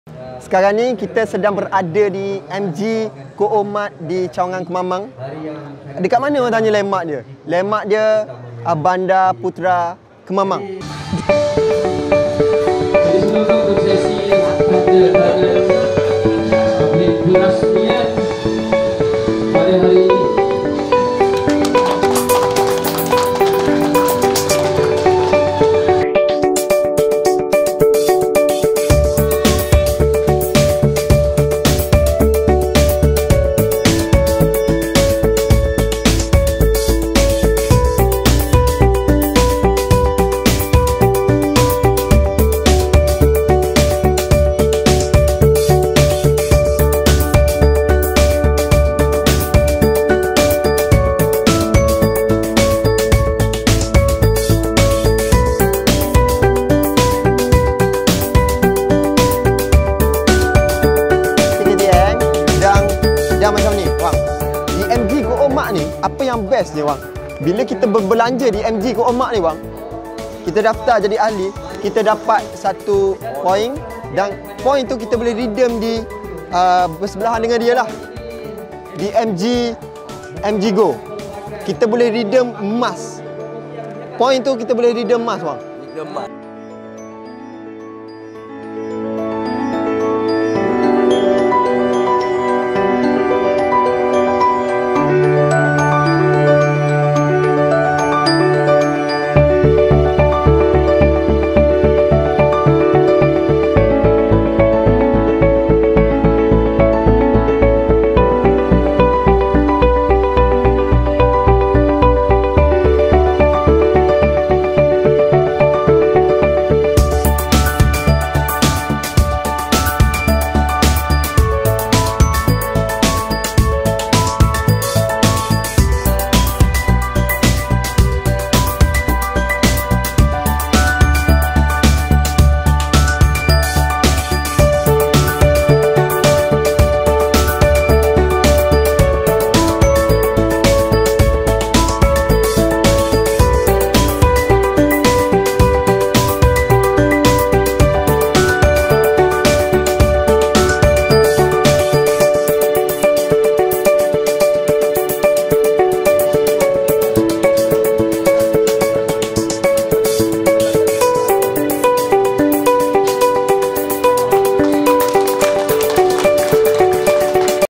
Awang mari kemaman
jom pakat serbu di MG Coopmart HQ kemaman di bandar putra kemaman...
kalau gi beli barang dapo situ sek awok boleh dapat poin,tapi kene daftar ahli lu ye kalau nok poin,dan poin tu boleh tebus dengan emas di @makmurgold.
macam2 ade situ...jom bowok anok branok pakat gi blake seme...